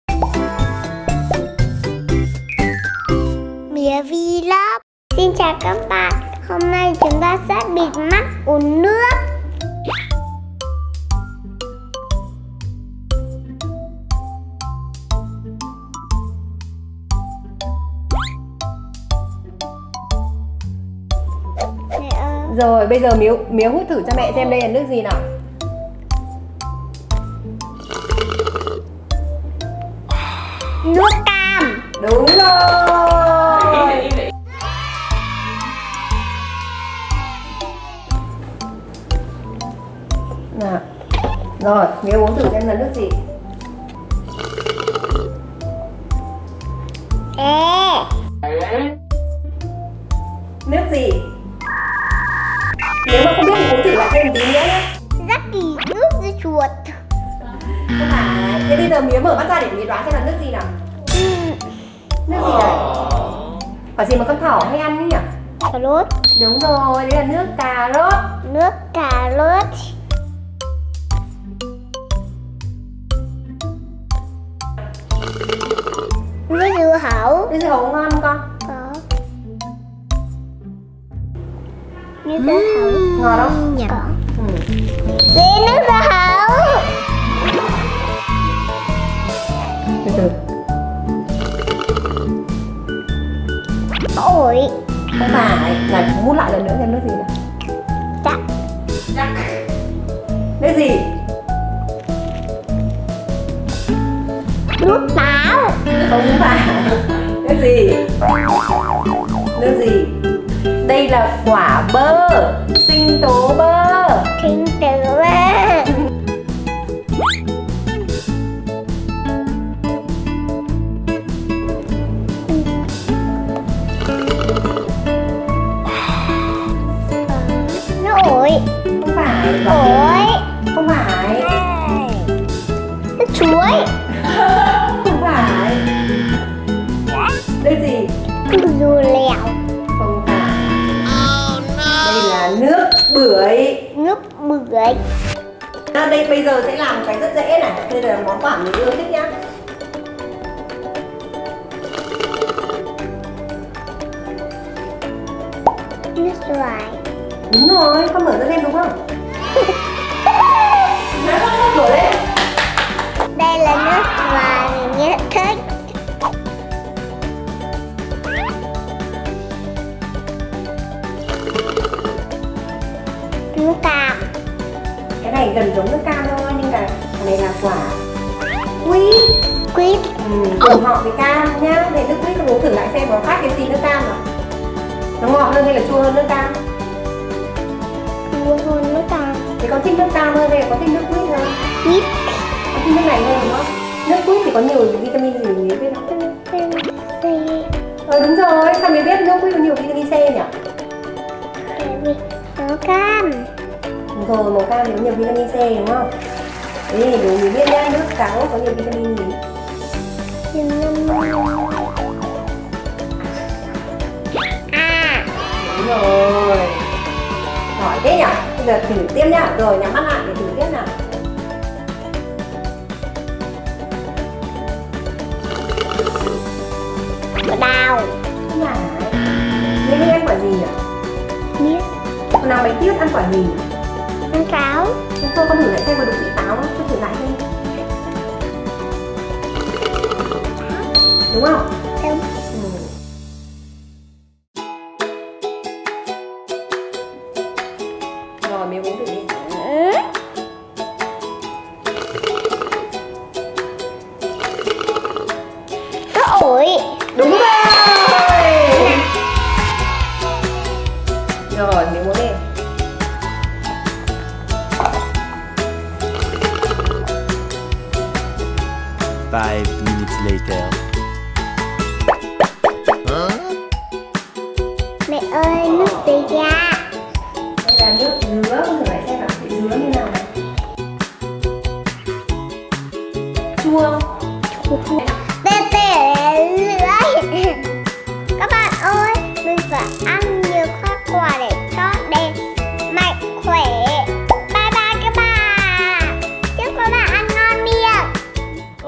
Mía Vlog | Nước này là nước gì? Trò chơi bịt mắt đoán nước mùa hè siêu vui
Mùa hè nóng nực thế này mà có được cốc nước mát uống thì đúng là siêu đã đúng không? Nhưng uống bình thường thôi thì dễ quá. Rủ bạn bè chơi trò bịt mắt đoán nước này để xem ai mới thực sự là thánh nước nha.